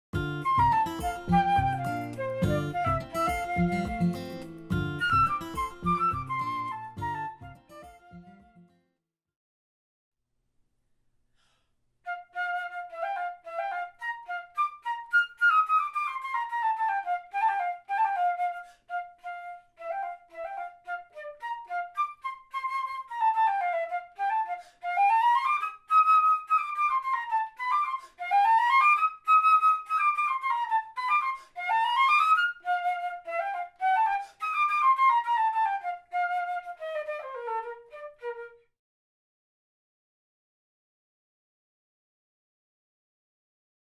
2020-2021 Florida All-State 7th-8th Grade Band Flute Technical Excerpt
Dr. Karen Large, flute professor at Florida State University, demonstrates the 7th-8th grade technical excerpt.

for a teaching video and handout for this excerpt.